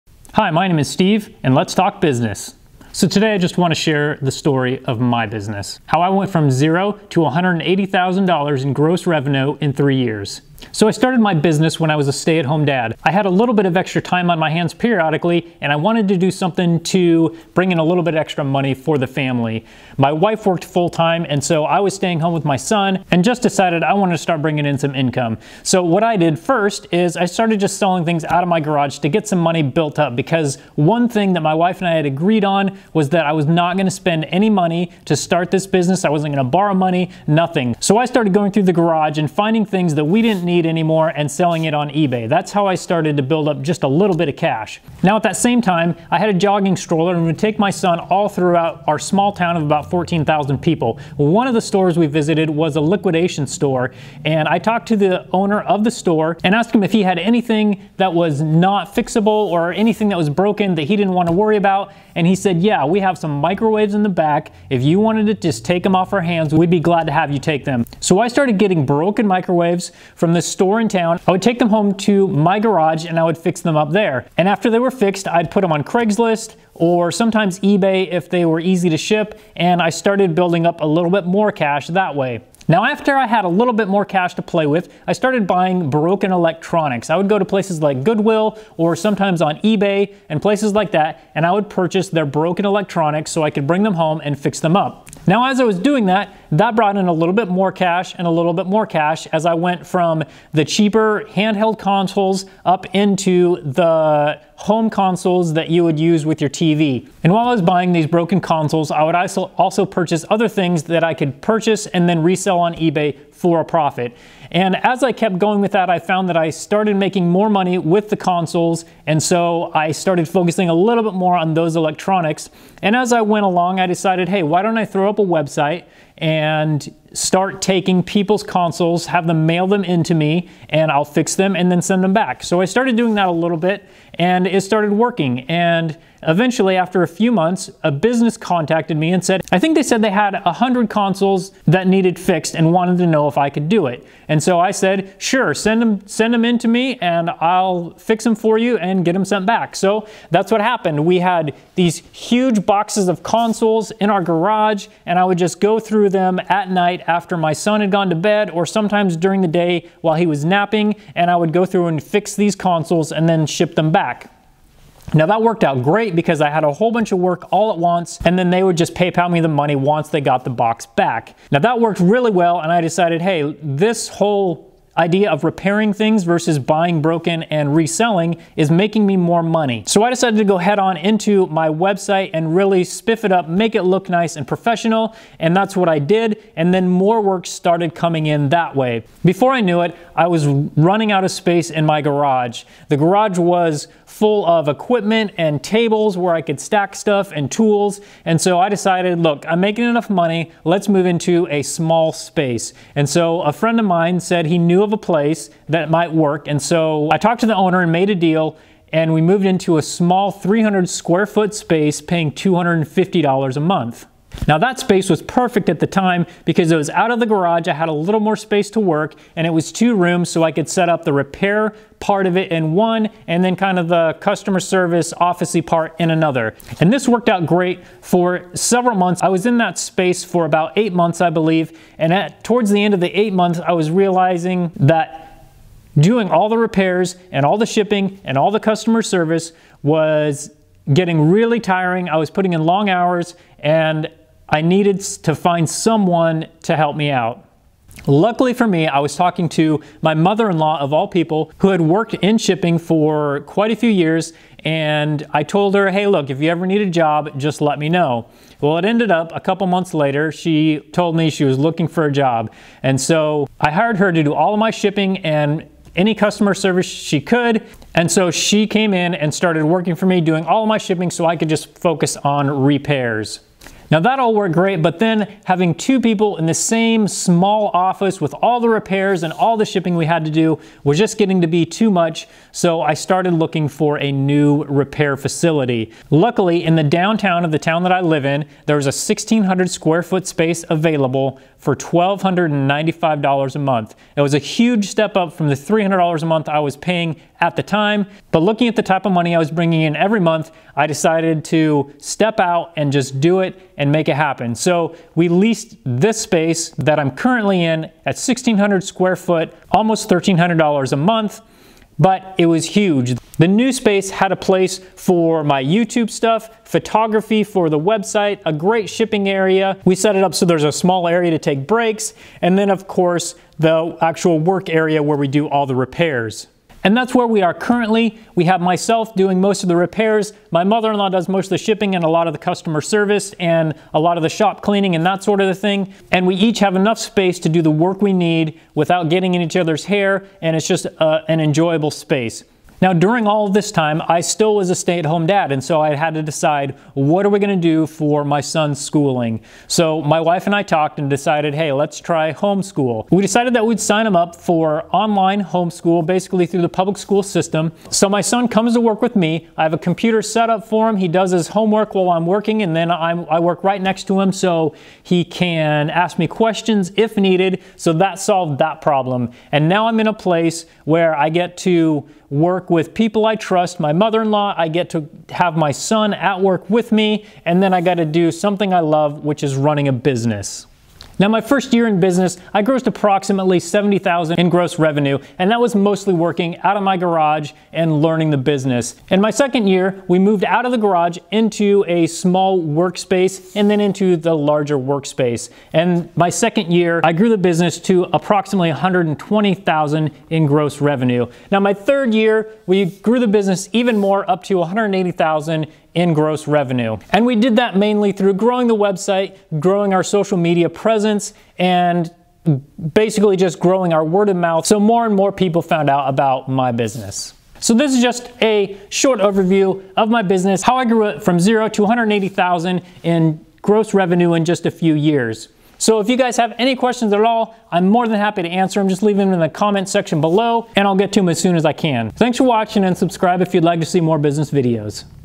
From $0 to $180K - How I Grew My Game Console Repair Business
In this video I share my business story. How I started my business with no money and grew it into a $180K gross revenue business in 3 years.
I started the business while being a stay at home Dad just to earn some income and contribute financially to the family. I started by sellingstuff out of our garage, moved to fixing broken microwaves, then into buying and selling Goodwill items, then to repairing broken electronics.
I'm happy to answer any questions you might have.